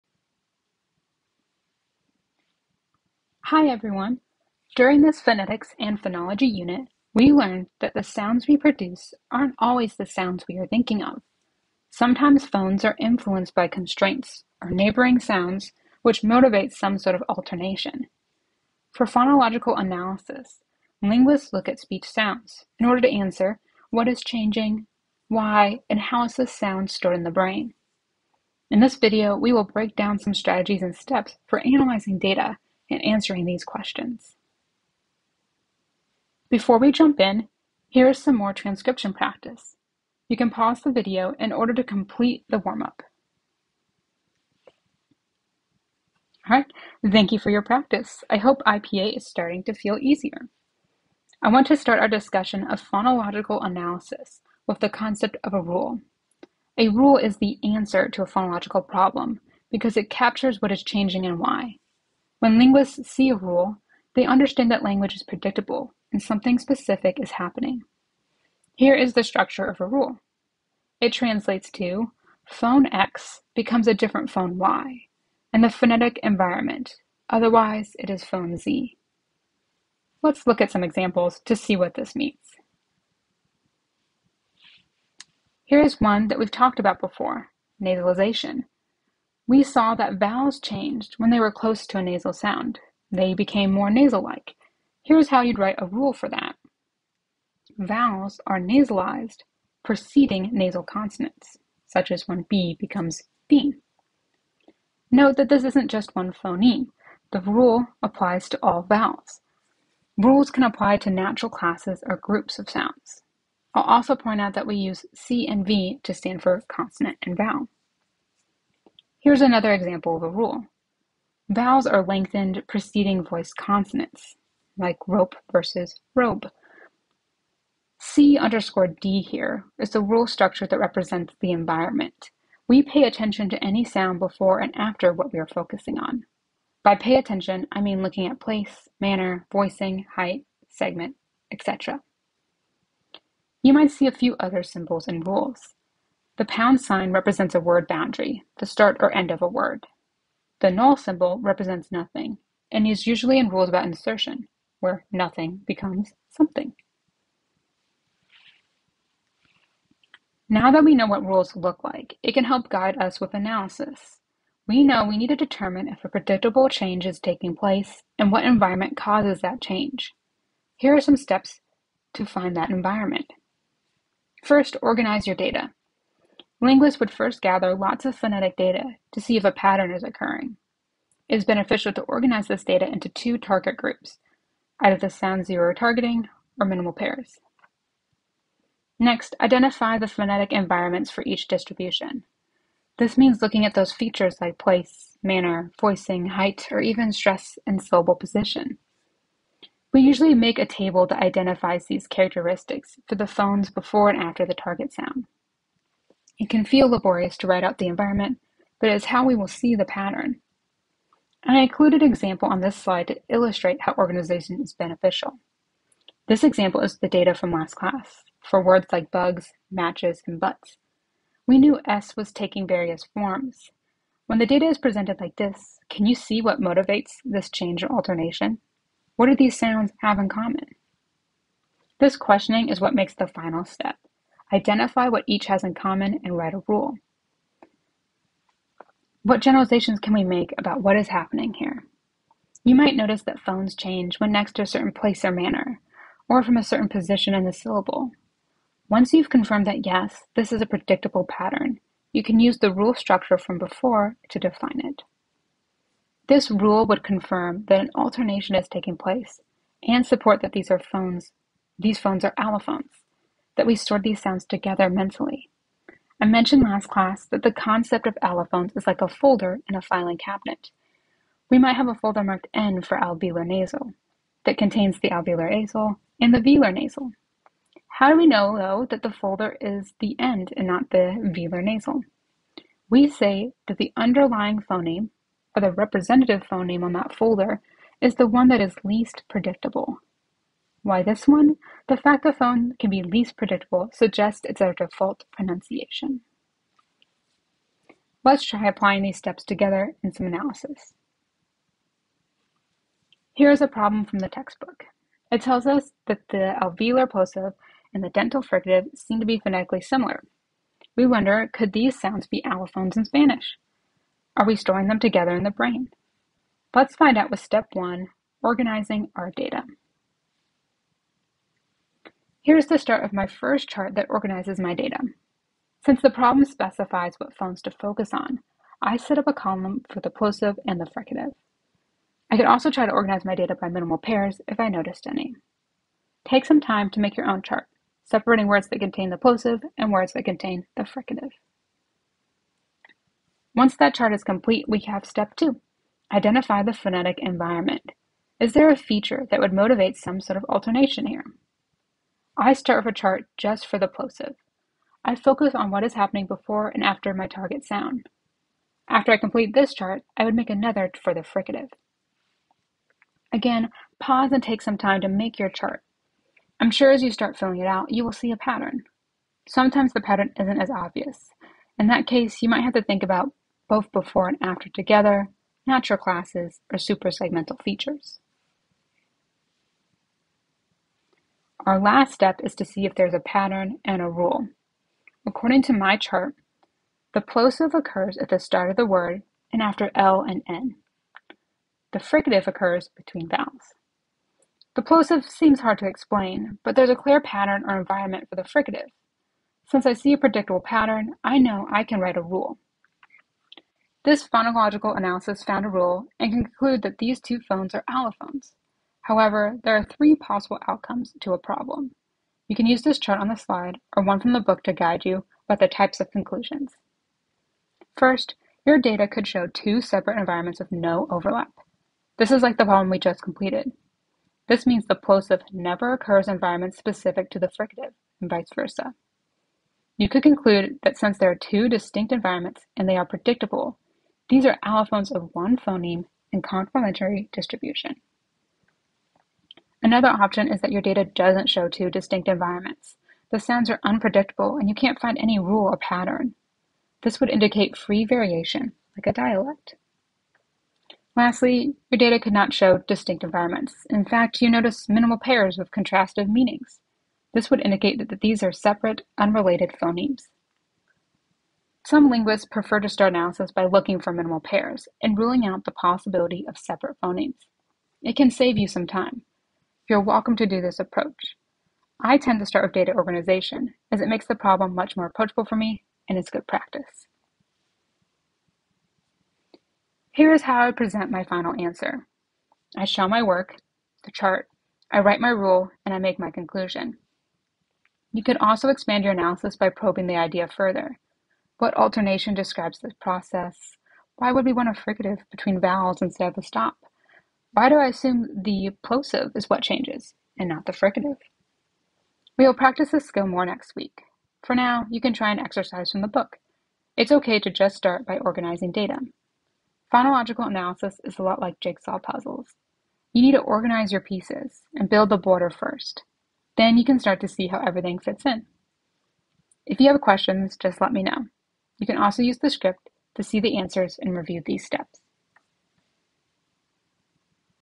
ENG219 - Steps for Phonological Analysis
This video covers how to analyze data and write a phonological rule (File 3.5). A script for this video can be found on Canvas. Timestamps are below. I believe I made a few slips and said phoneme when I meant phone. My apologies!

00:00-00:35 Introduction
00:36-00:45 Transcription Warm-Up
00:46-01:22 What Rules Look Like
01:23-02:38 Examples of Rules
02:40-05:01 Steps for Finding a Rule
05:03-05:20 Problem from the Book
05:21-06:20 Steps 1 and Step 2 (Organizing Data and Identify Environment)
06:21-07:58 Step 3 (Find Rule and Make Conclusion)
07:59-8:48 The answer and wrap up